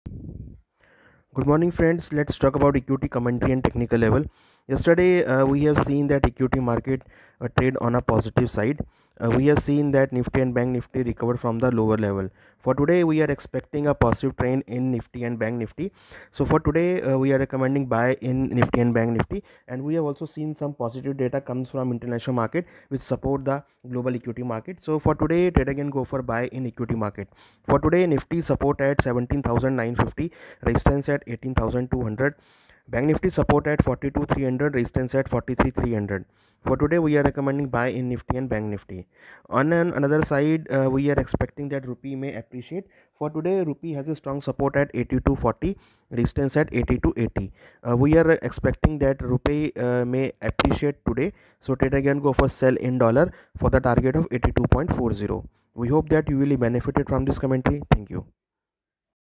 Daily Market Outlook 27.12.2022 by Mr. Anuj Gupta, IIFL securities
SEBI Registration number is INM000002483